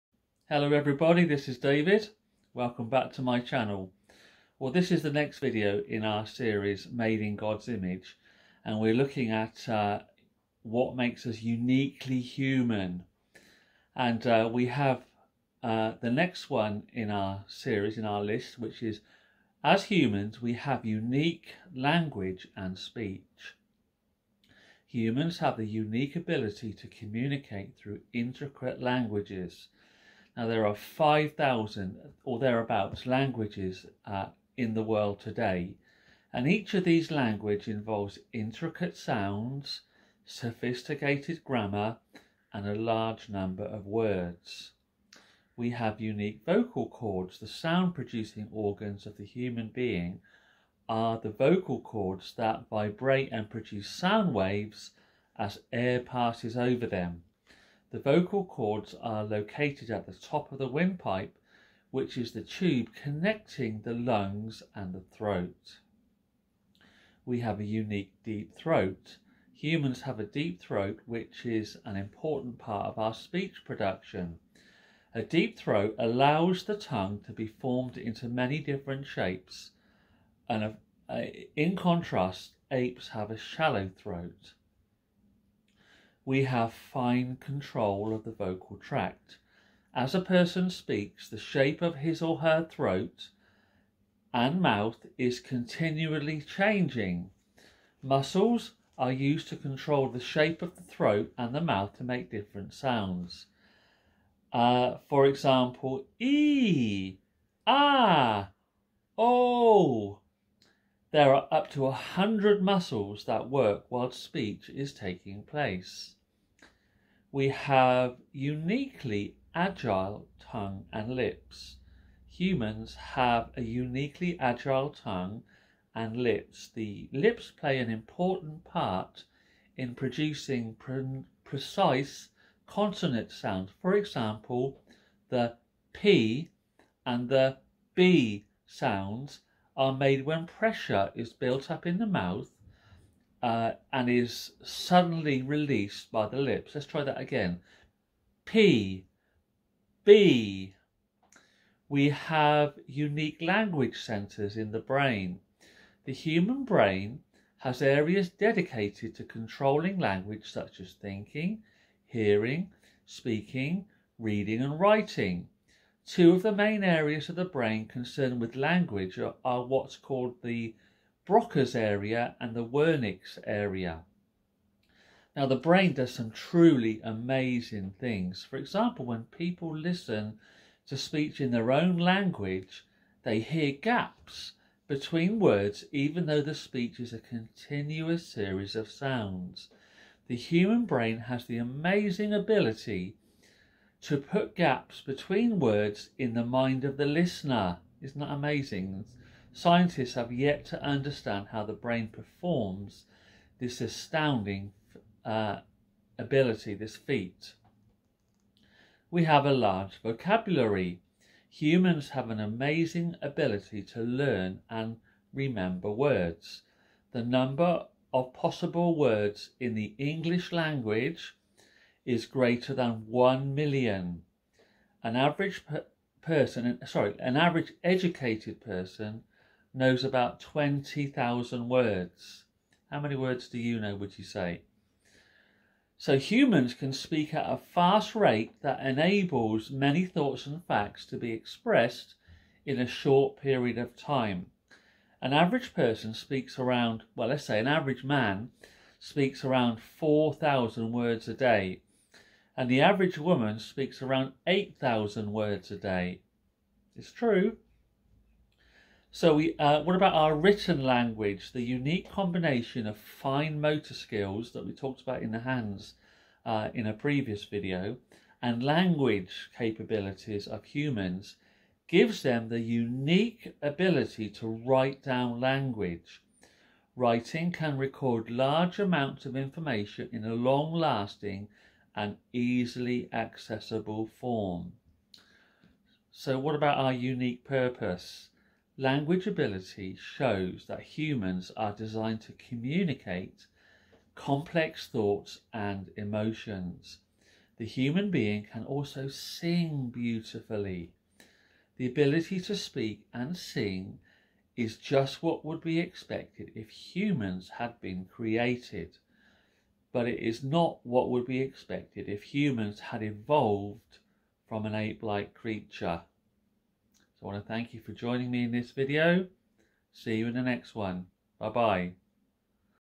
Made in God's image. Uniquely human. Our unique language and speech.
As human beings we have the unique ability to communicate through intricate language.